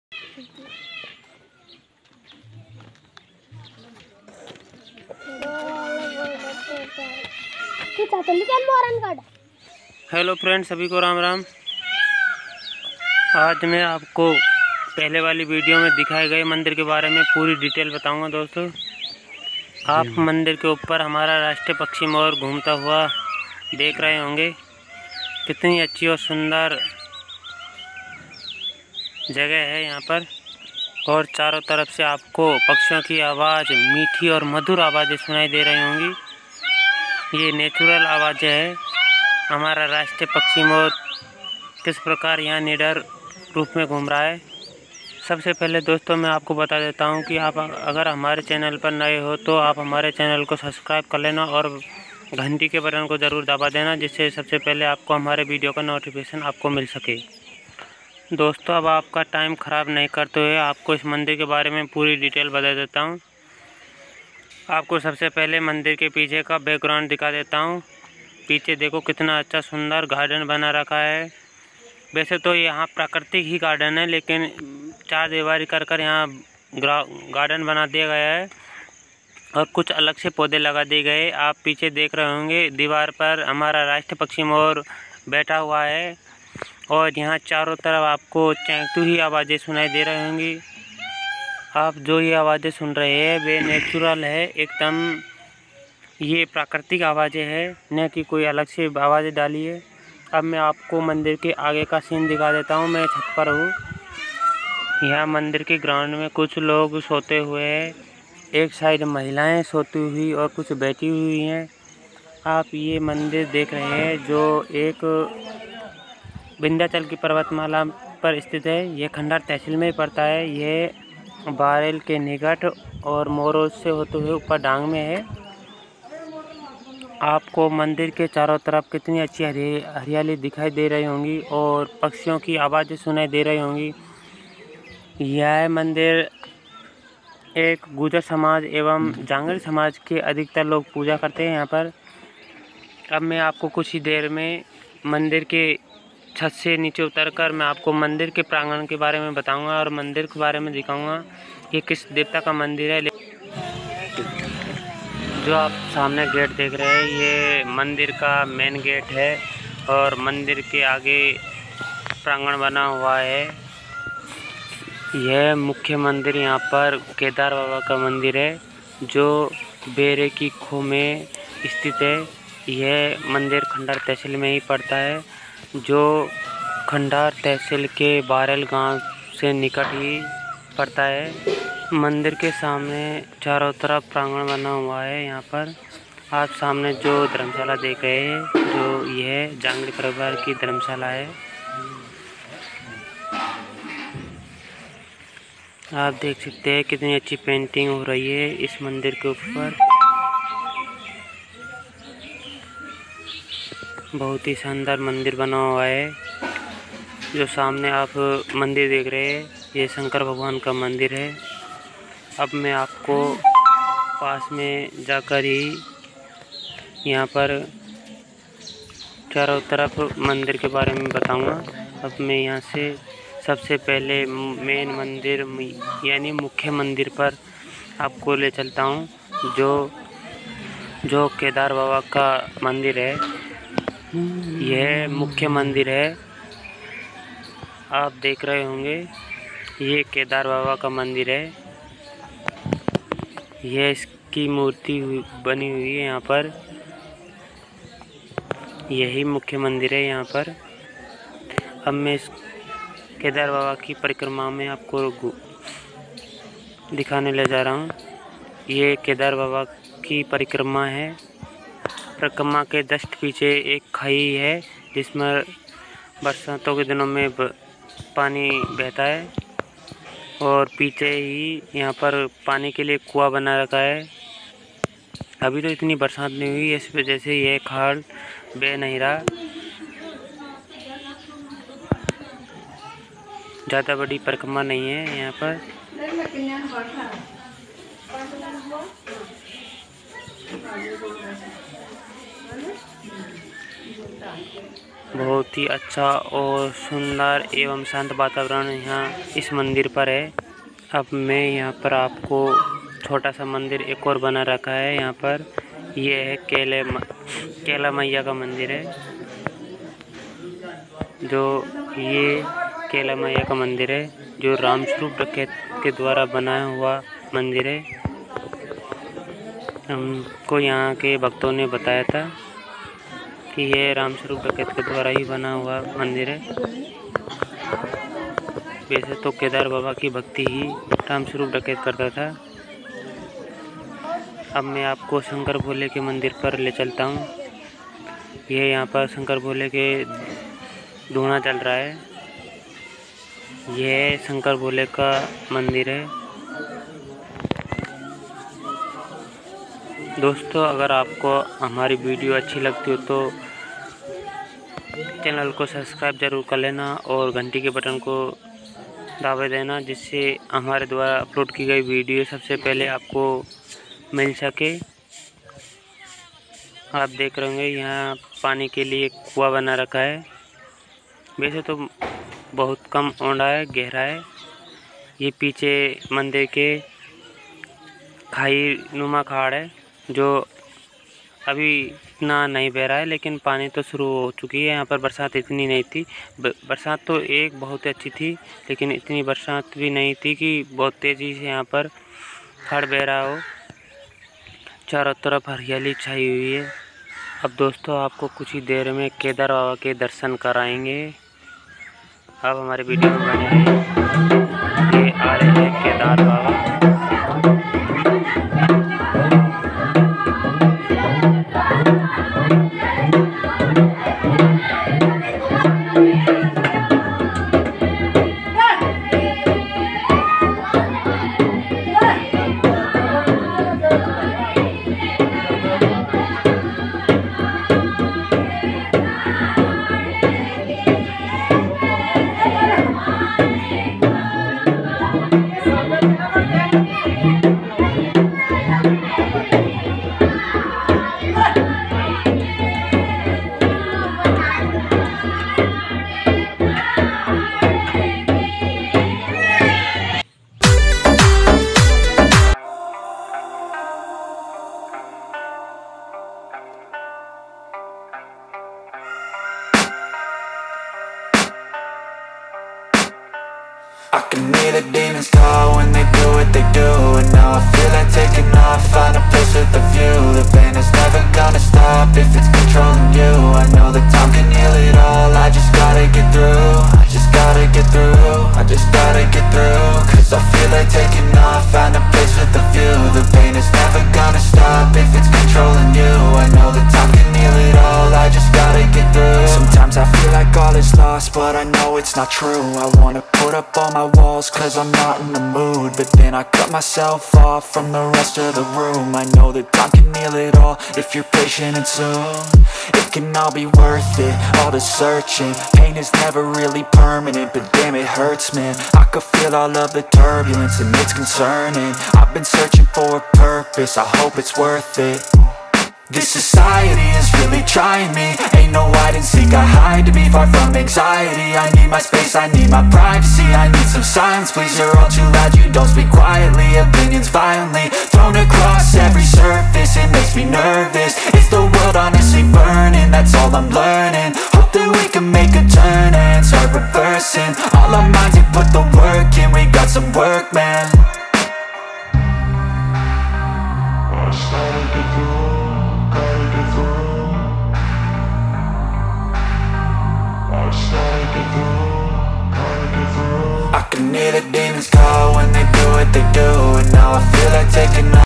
बेरई की खो !! रामस्वरूप डकैत का शरण स्थल !! मुख्य मंदिर केदार बाबा का मंदिर ,,,
बेरई की खो में केदार बाबा का मंदिर